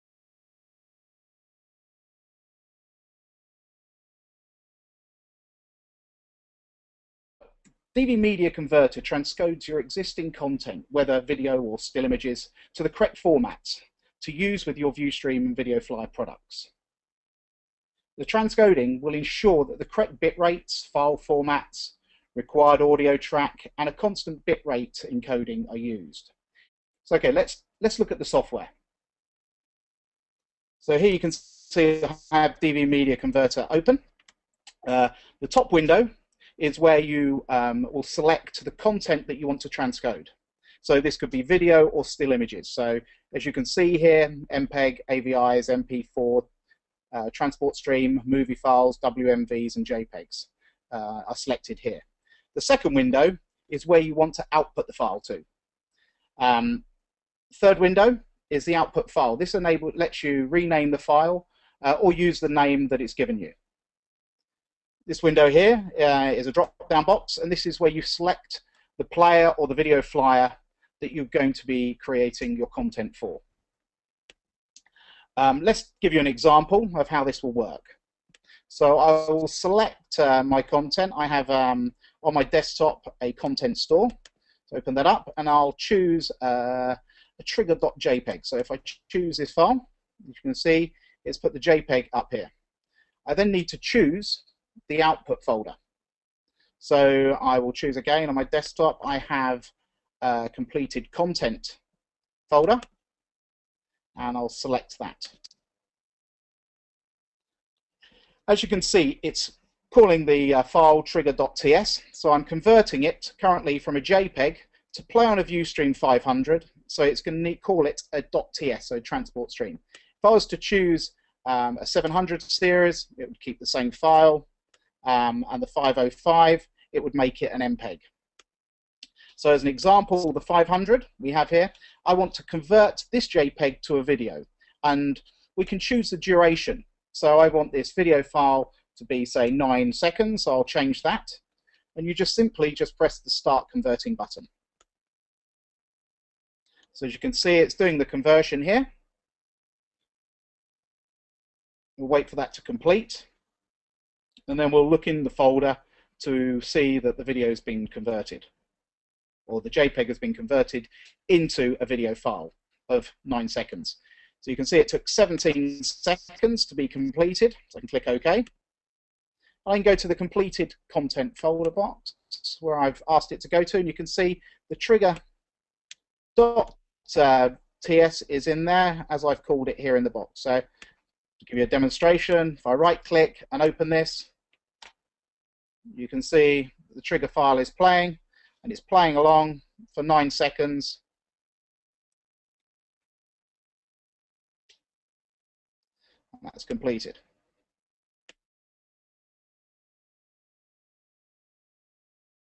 DV-Media-Converter Software
A quick look of the DV Signage Media Converter Software.